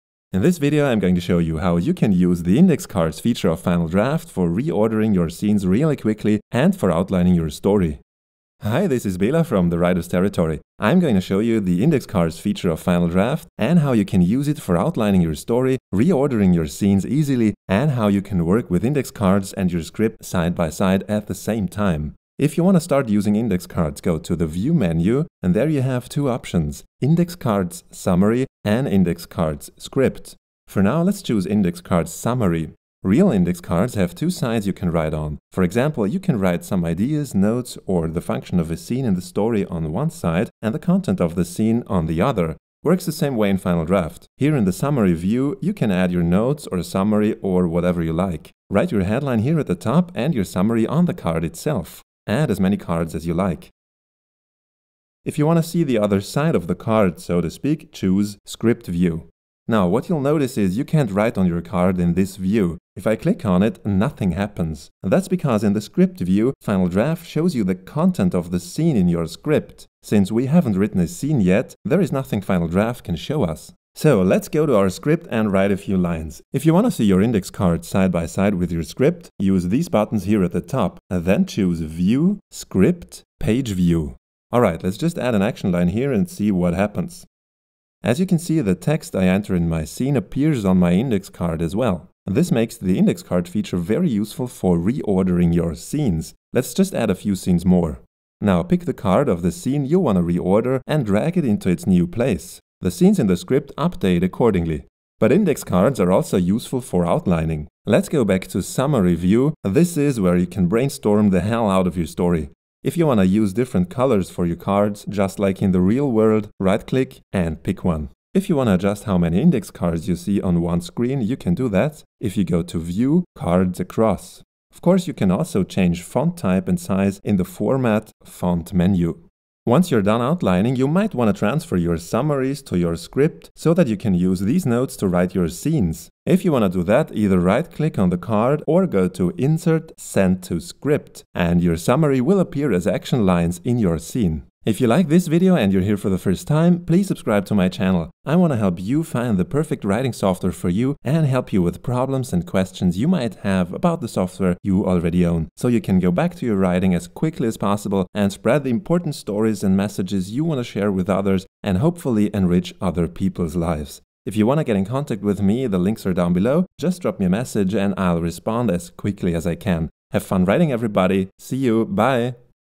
Final Draft Tutorial: Index Cards: Reordering Scenes and Outlining your Story
⬇️ ⬇️ ⬇️ FREEBIES for Writers:

➡️ Free Story Structure Cheat Sheets (Hero's Journey, Save the Cat, Mini Movie Method):